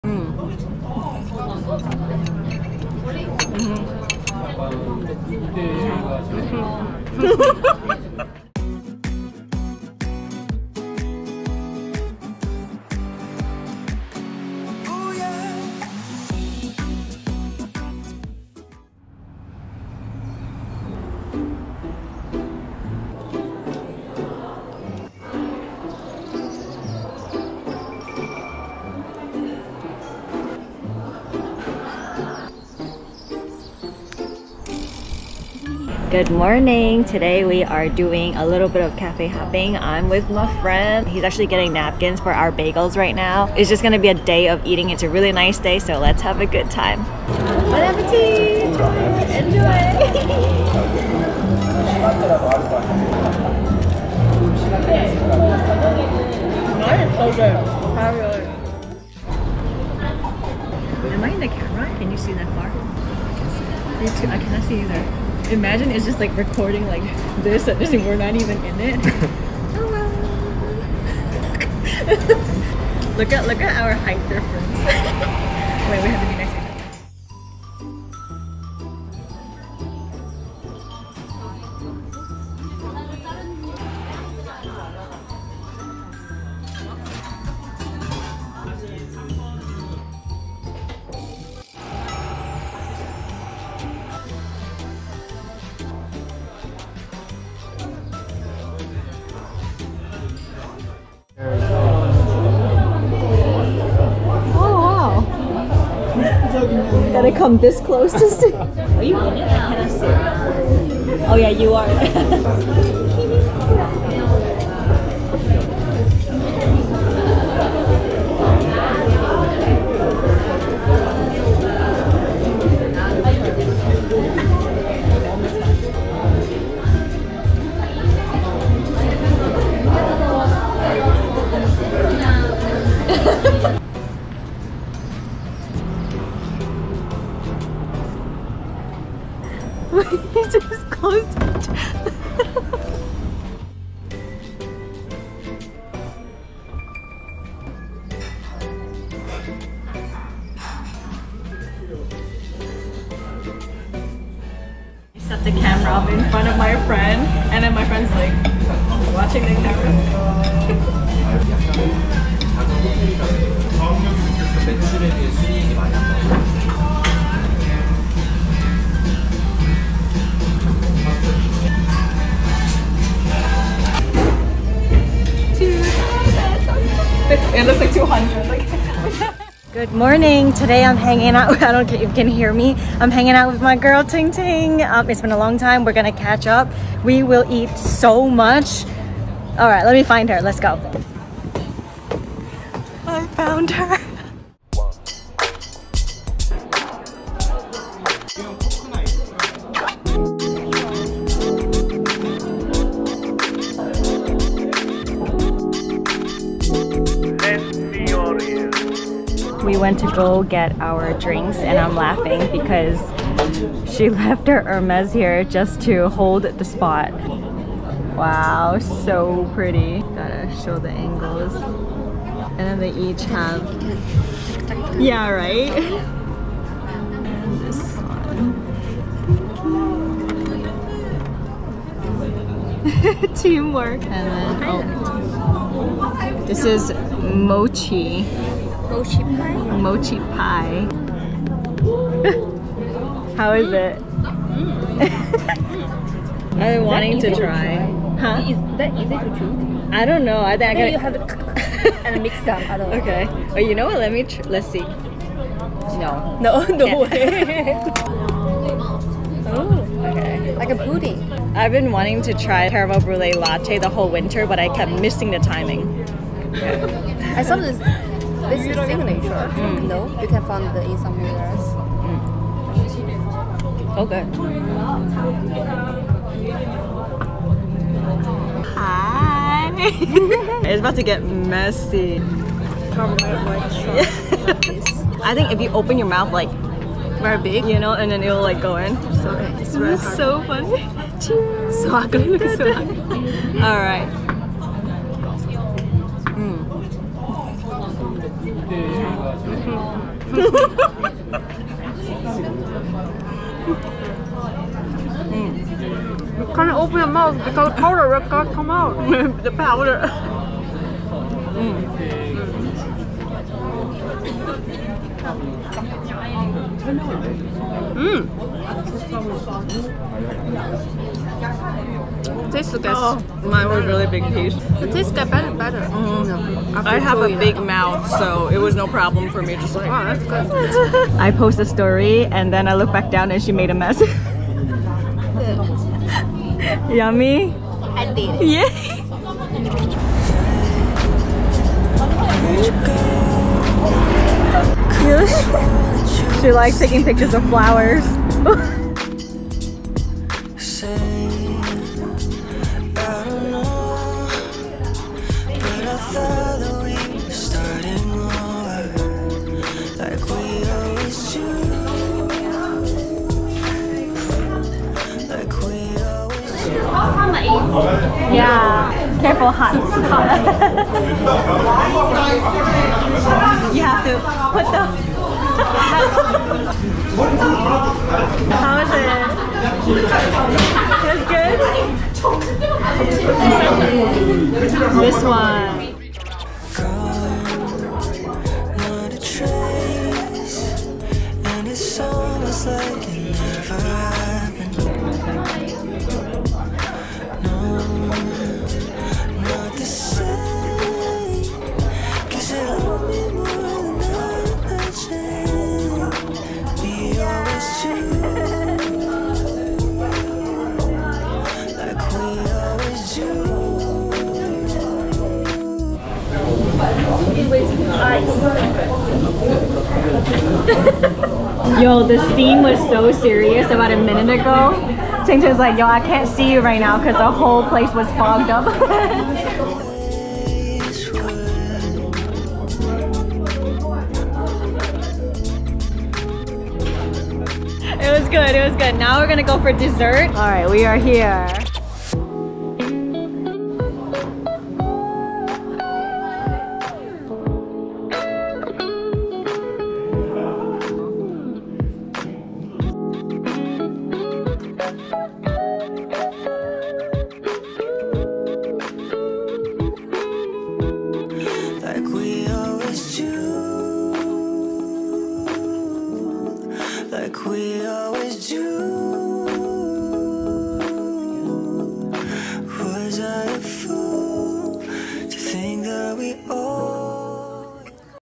hangout with me for a weekend | realistic vlog
THANKYOU 2K like yall the best for continuously hanging out with me 🙌 appreciation dance post tomorrow!

🍽️ : Toda Bagel
🍽️ : Peuritcheu
🍽️ : Mennoaji
🍽️ : Pie In The Sharp
🍽️ : Chowon
🍽️ : Salty Cabin

🎵 : @ Campsite Dream
🎵 : @ RFM / ForArtist
🎵 : @ Keshi

And the instafam :
@ vlap2t
@ tingting57live @TingtingASMR

🔍 p.s. all searches works best via navermap